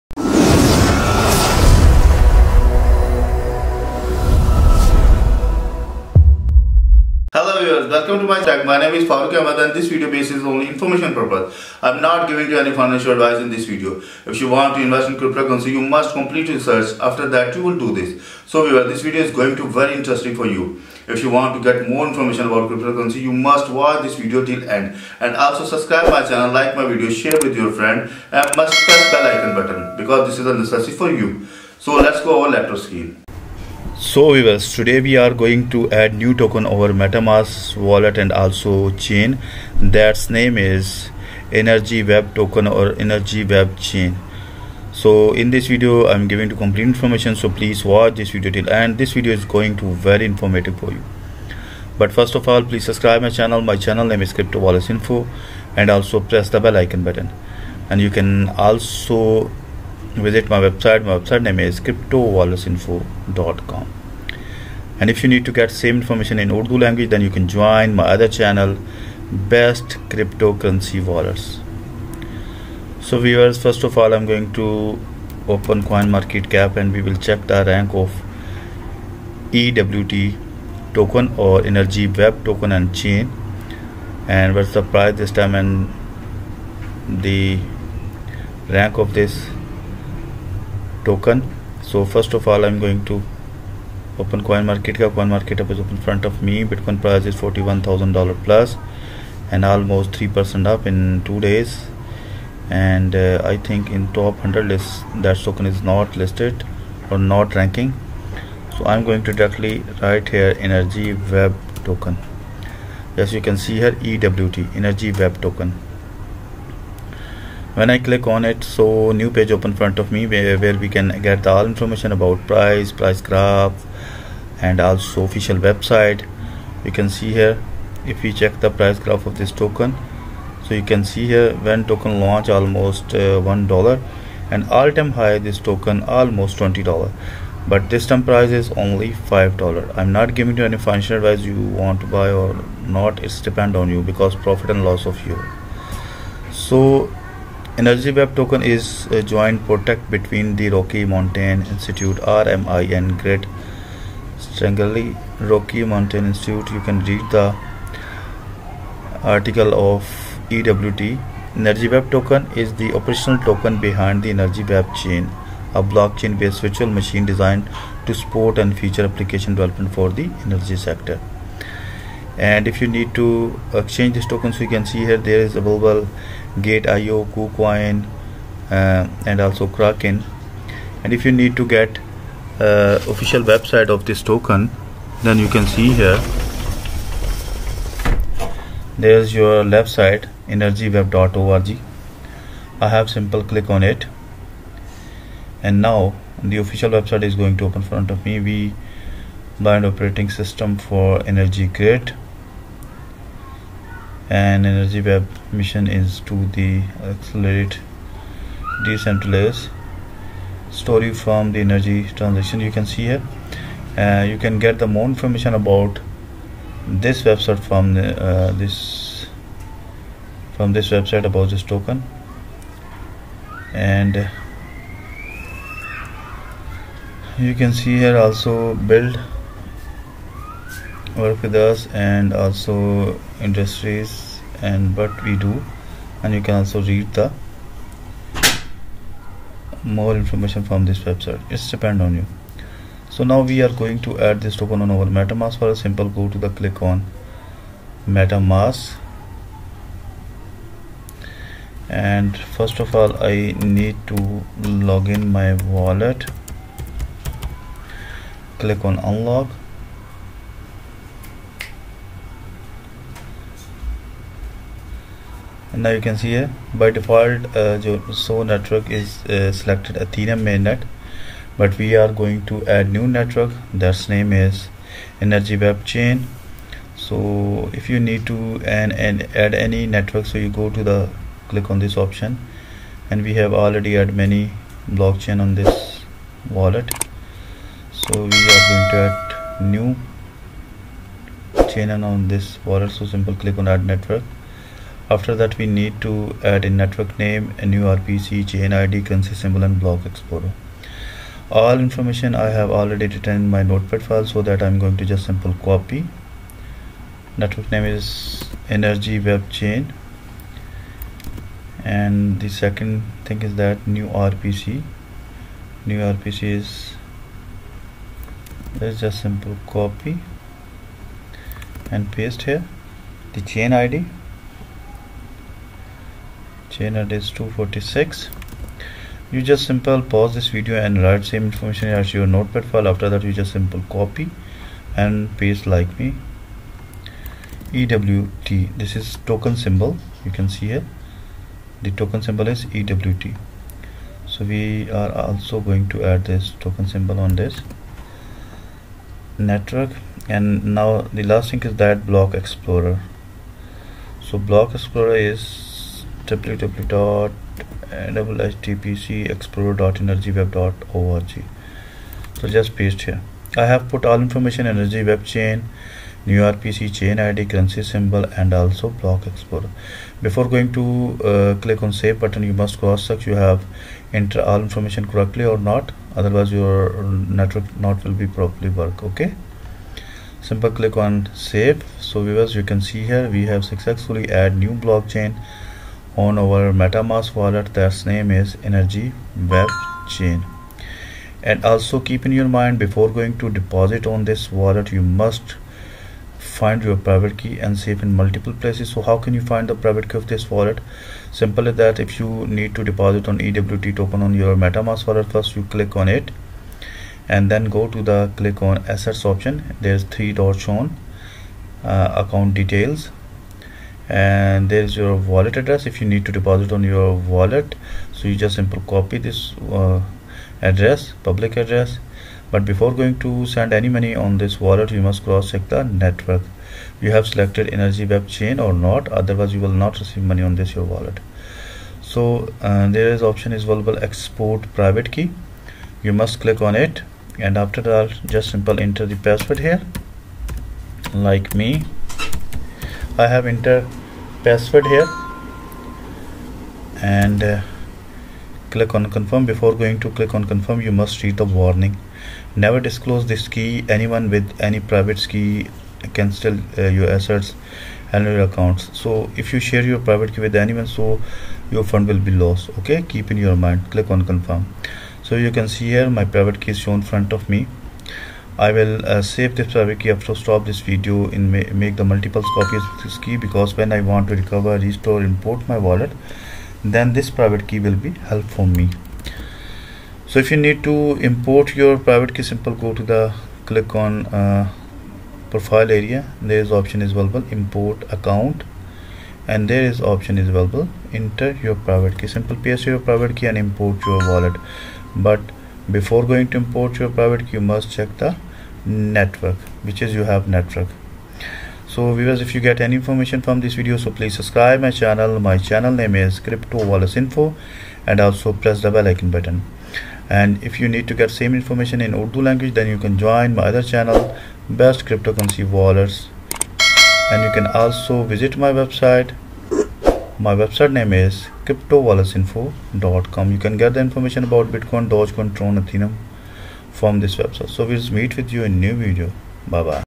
How to Add Energy Web Chain To Metamask | Energy Web Token | Crypto Wallets Info
How to Add Energy Web Chain To Metamask. Energy Web Token (EWT) is the operational token behind the Energy Web Chain, a blockchain-based virtual machine designed to support and further application development for the energy sector.

Energy Web Chain was launched in June 2019. The Energy Web Foundation is the non-profit enterprise behind the project. EWT aims to bring diversity to the energy sector by allowing developers to create decentralized applications (DApps). The virtual machine has the potential to benefit actors from all areas of energetics, including grid operators, software developers, and vendors. The MetaMask app is both a wallet & a browser. Buy, send, spend & exchange your digital assets. Make payments to anyone, anywhere. Log into websites securely to trade assets, lend, borrow, play games, publish content, buy rare digital art, and so much more.